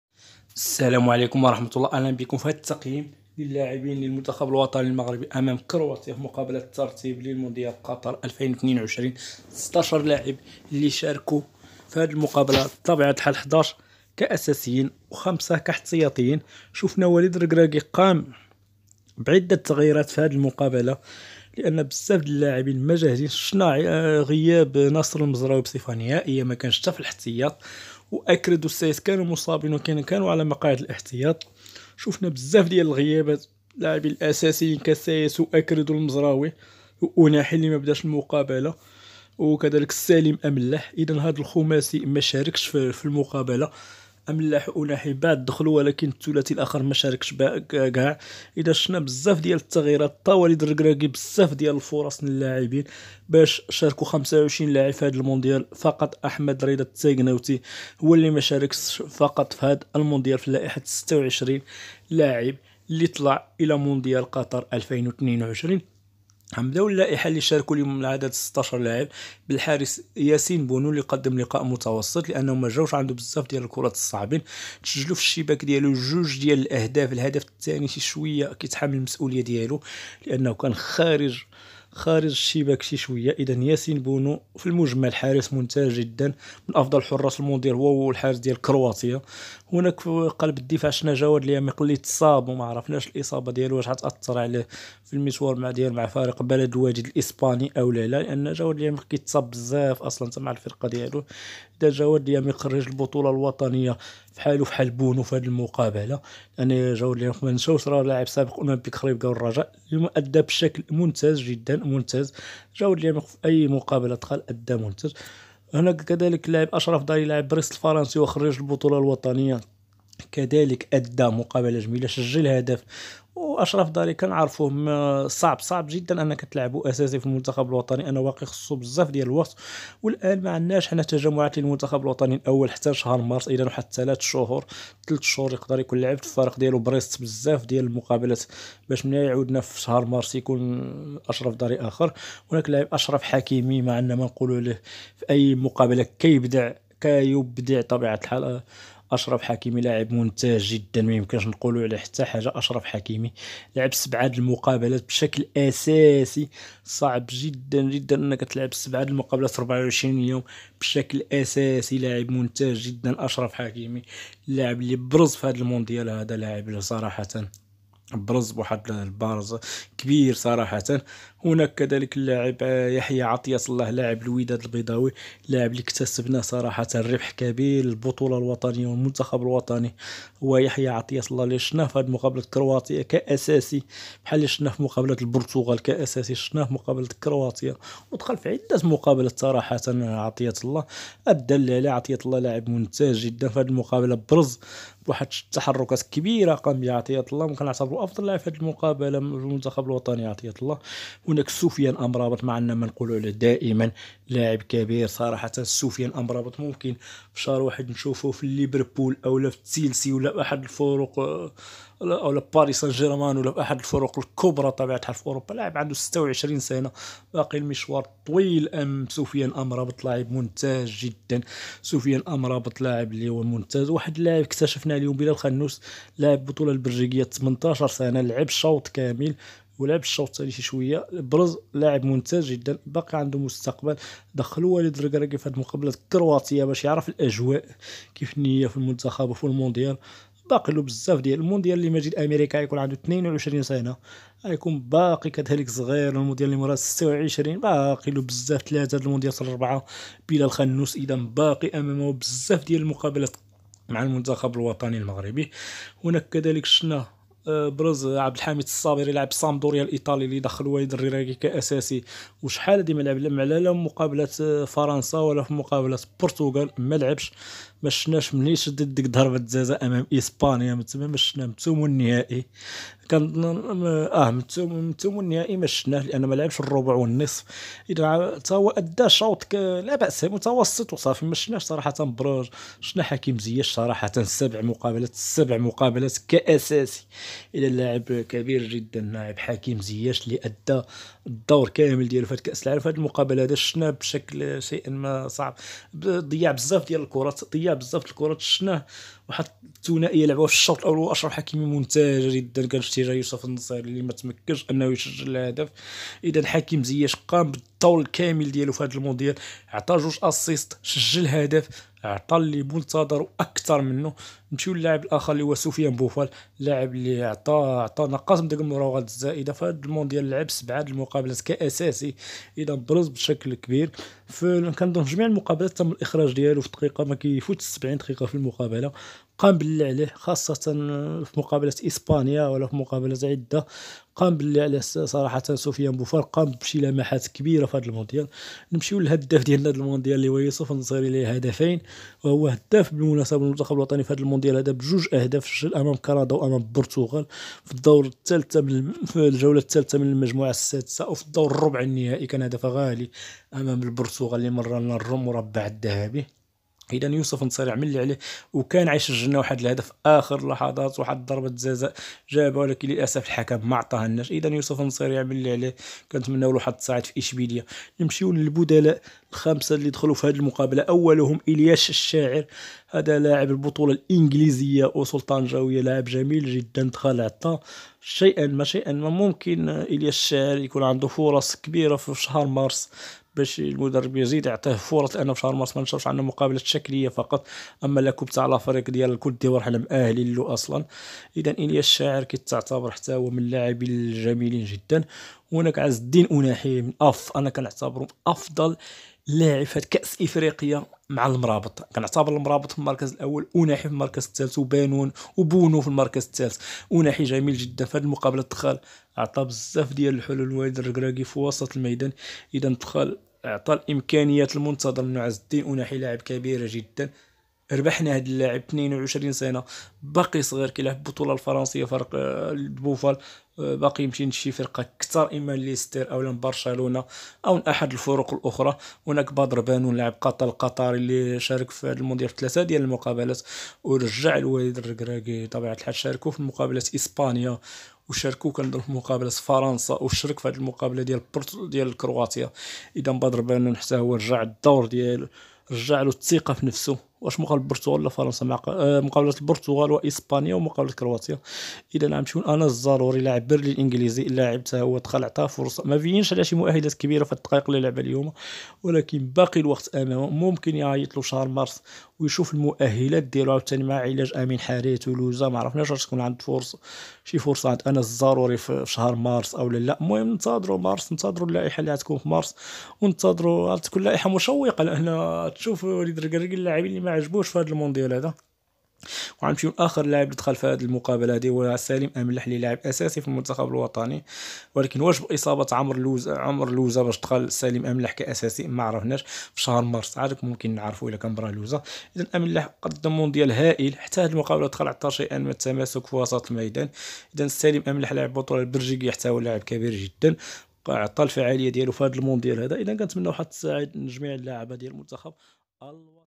تحليل اداء مستوى لاعبين المنتخب الوطني المغربي امام منتخب كرواتيا في مونديال قطر 2022
تحليل اداء لاعبين المنتخب الوطني المغربي امام منتخب كرواتيا مونديال قطر 2022